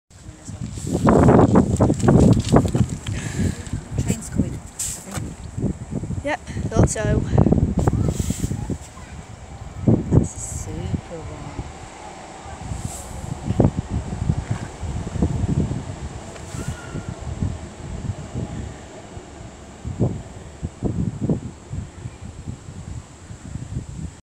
Sunny day at the Aquaduct (and viaduct)
I went to Marple Aquaduct hoping to see some trains. So I took my tablet to take pics. Then, right on cue, a train comes over the Viaduct. What are the chances?